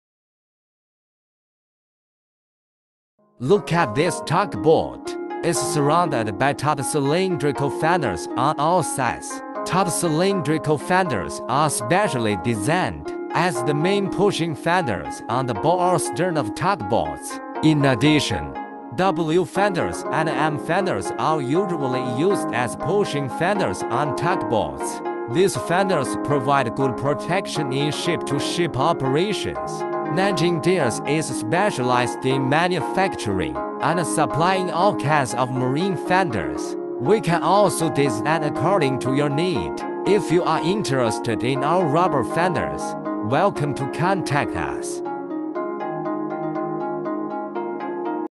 Pushing fenders used on tugboats
The rubberfenders commonly used on tugboats are mainly include tug cylindrical fenders, M fenders, W fenders and keyhole fenders.